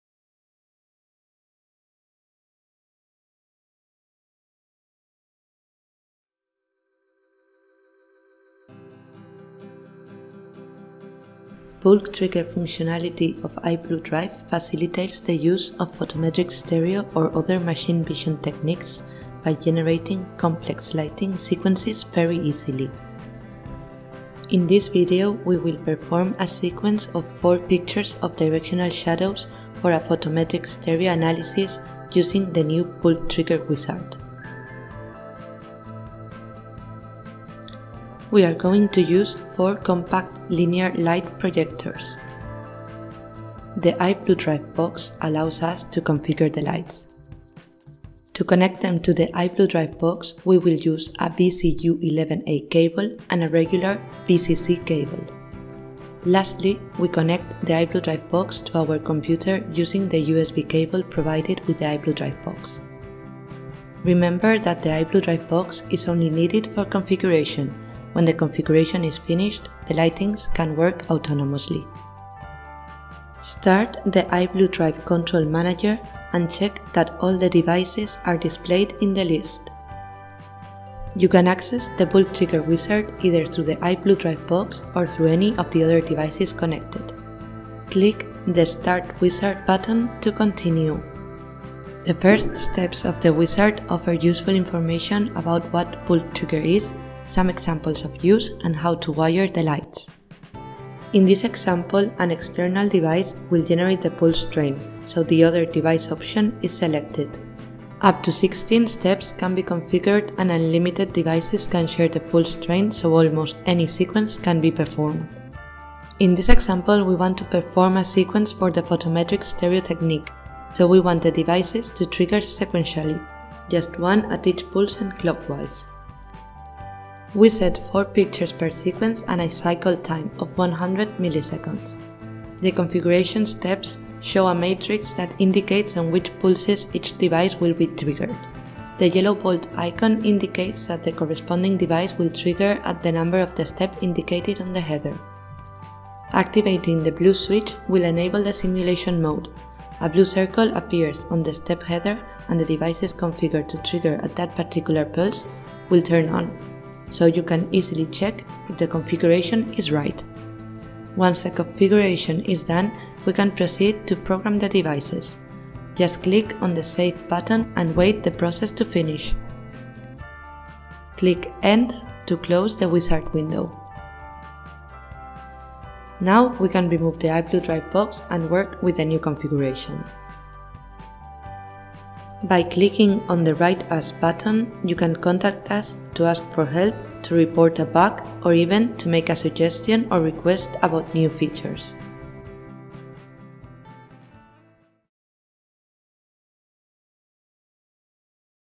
Bulk Trigger Wizard - iBlueDrive CM
DCM Sistemes
Bulk Trigger Wizard included in the latest release version of the iBlueDrive Control Manager.
Products used:
4xPLC0824C/630i/AW